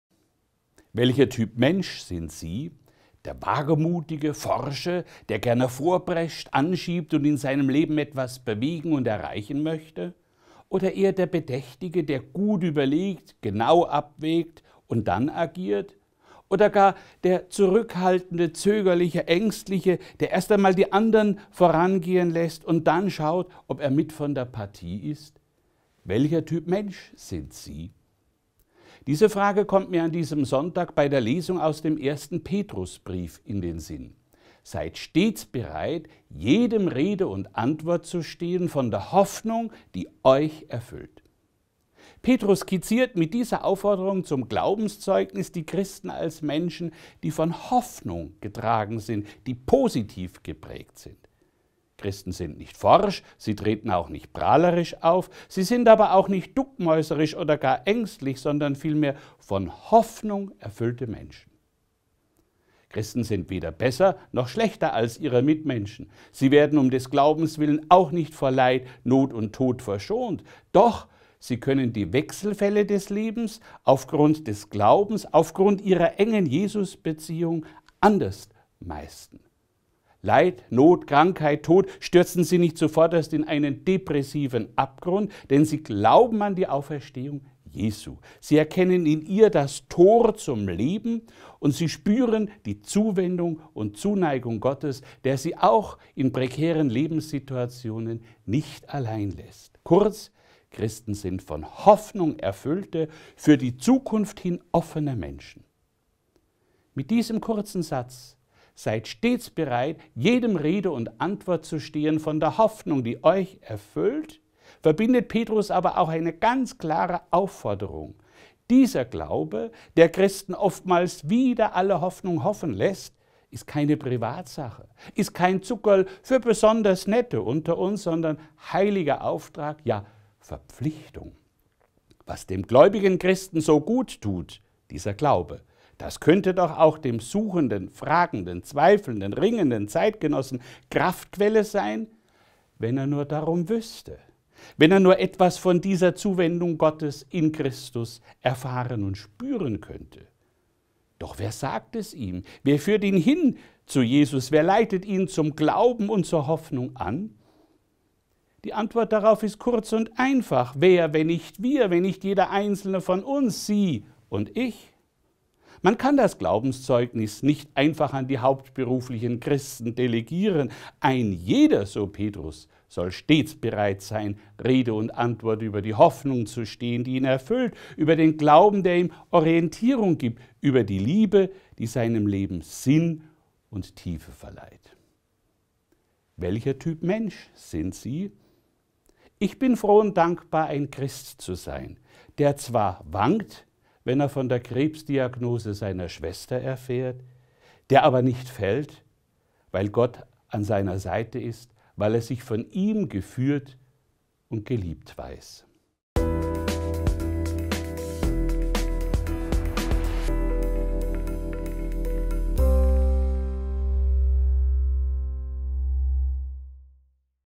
Christen haben Hoffnung - Dr. Bernhard Kirchgessner am 21. Mai 2017
Die Ausgabe der Sonntagspredigt zum 6. Sonntag der Osterzeit am 21. Mai spricht Dr. Bernhard Kirchgessner, Leiter des Exerzitien- und Bildungshauses Spectrum Kirche Passau.